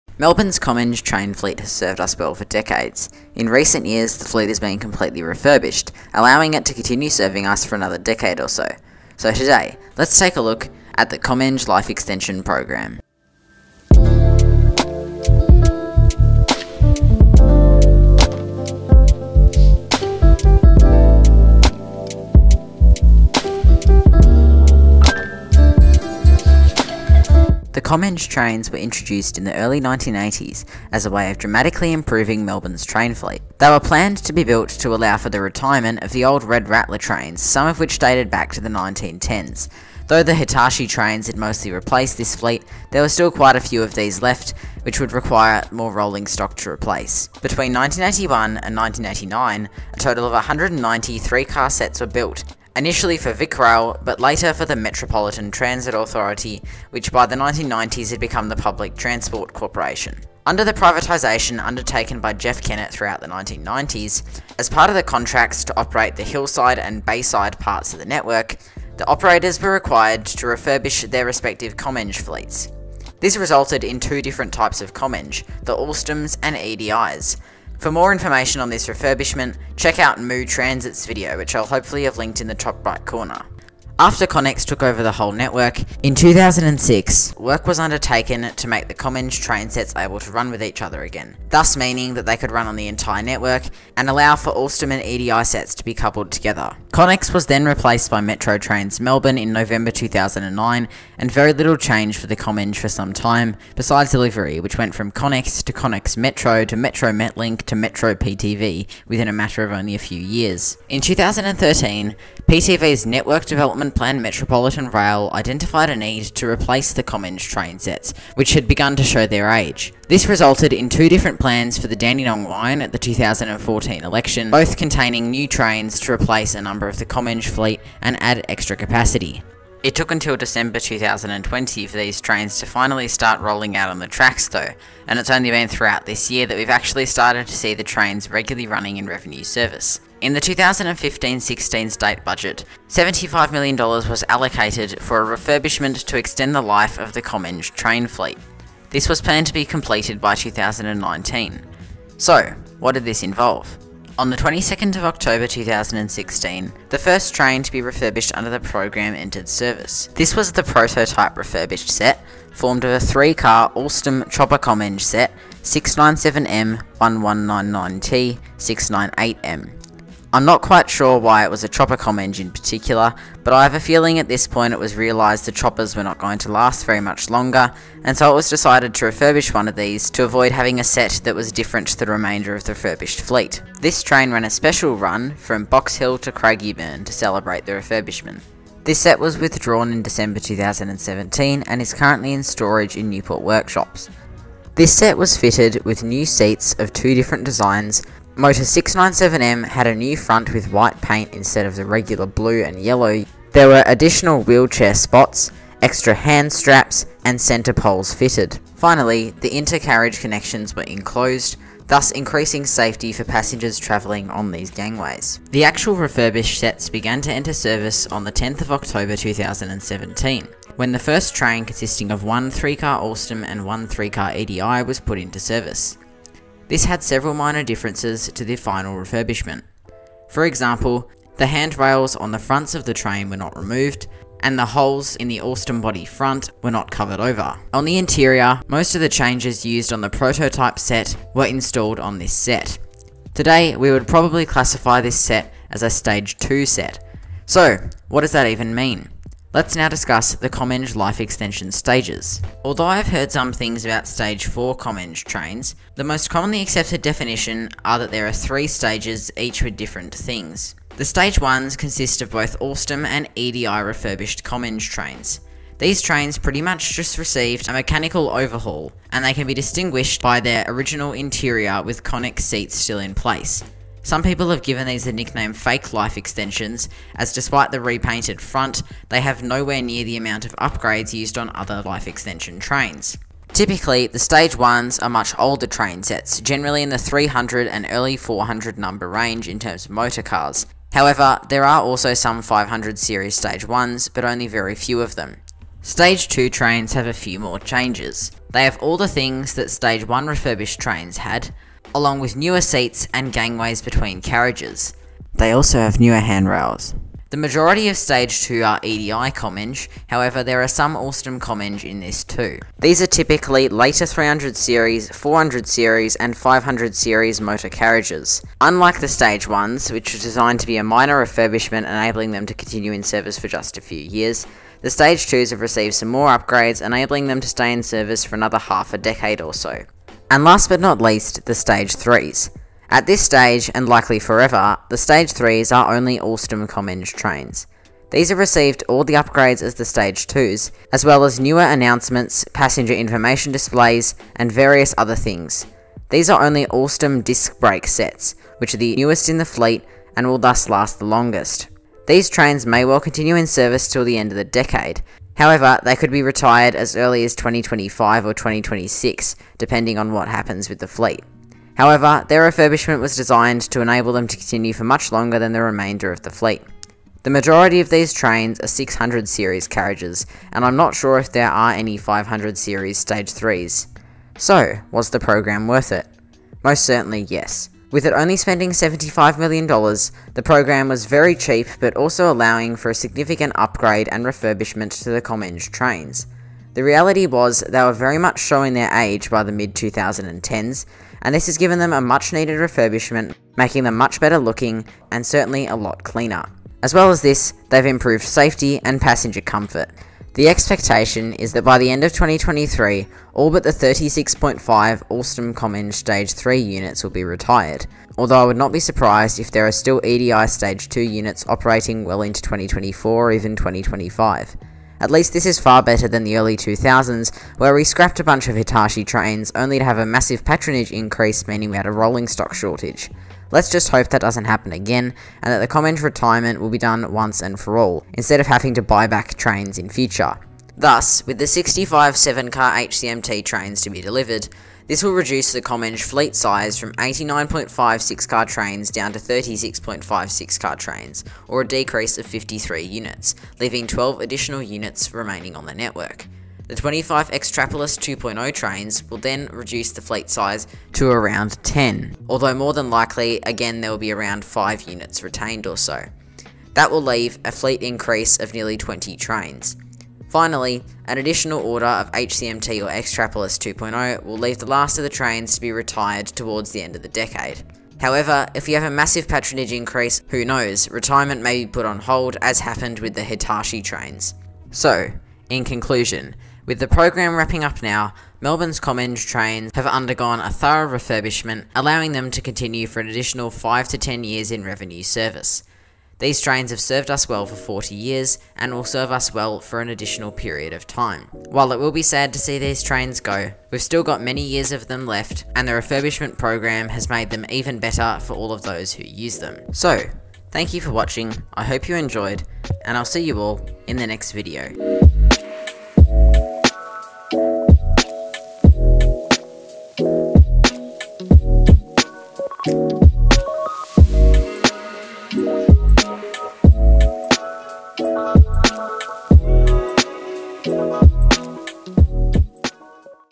The Comeng Life Extension Project
Melbourne's Comeng train fleet has served us well for decades. In recent years, the fleet has been completely refurbished, allowing it to continue serving us for another decade or so. So, let's take a look at the Comeng Life Extension project.

Chapters
00:00 intro
00:28 background history
02:34 The prototype refurbished set
03:36 First life extension set
04:12 Comeng life extension stages – stage 1
04:55 Comeng life extension stages – stage 2
05:30 Comeng life extension stages – stage 3
06:14 summary of the refurbishment
06:40 the future and retirement
07:54 conclusion

Credits
Videos and images either taken by myself or from Weston Langford Daniel Bowen or Marcus Wong